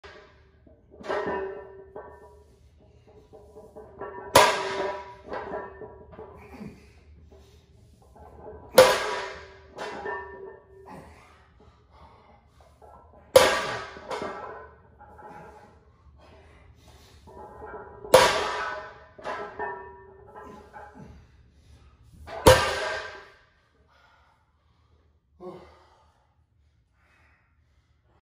Stiff Leg DL - Slow Tempo - 2” Deficit - 505lbs
Using a slow tempo and concentrating on the squeeze.  It makes them tougher but I feel the benefits of these.